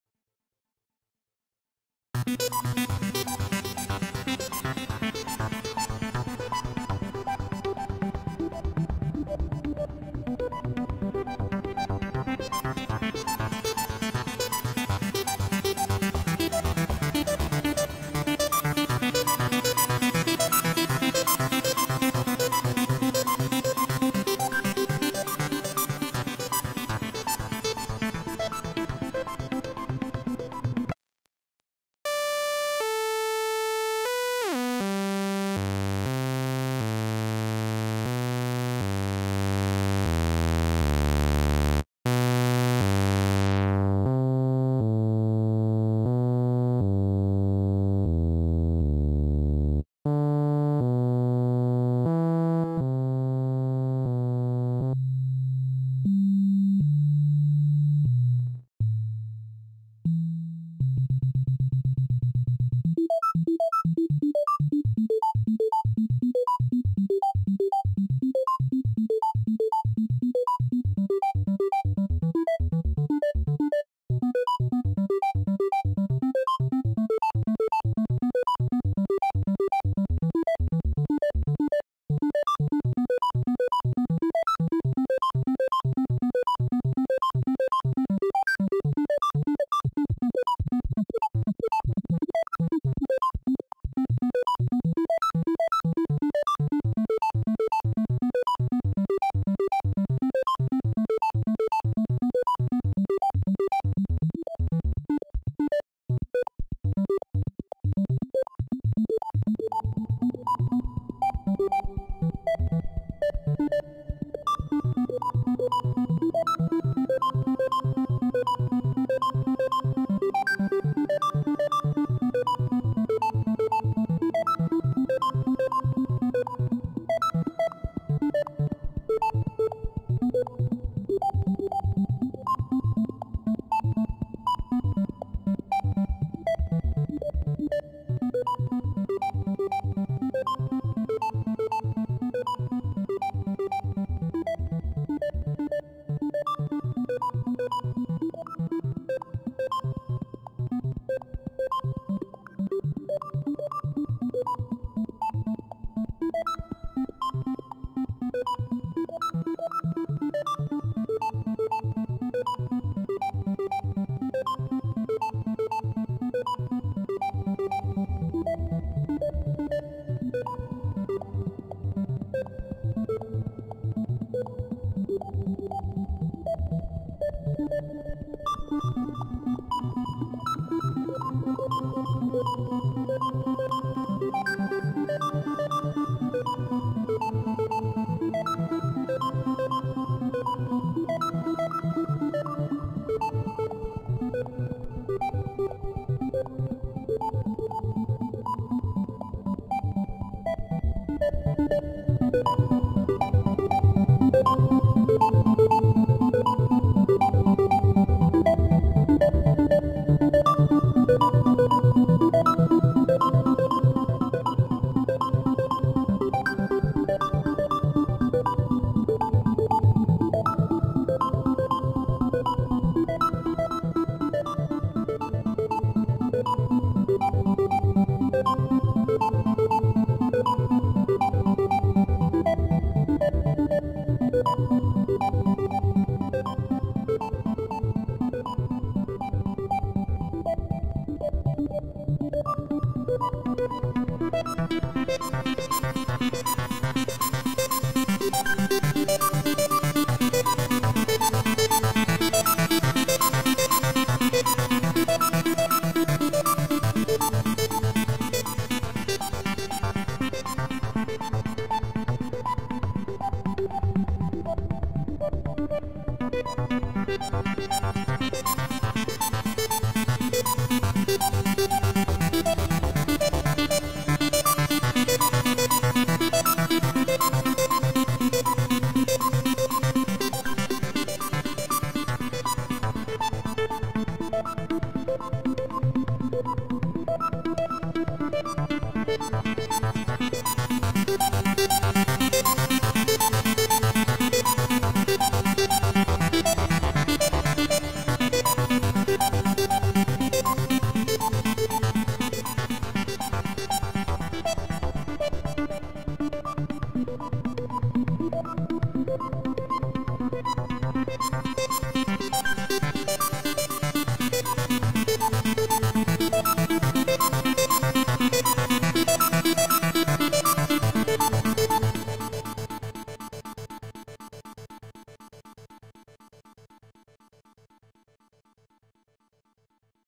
How To Make Acid Arp Sound On V Station Vst
This tutorial shows how to create acid arp sound in v station virtual synthesizer.

This preset included in v station sound bank with 1000 sounds designed by "Ruben Hymov":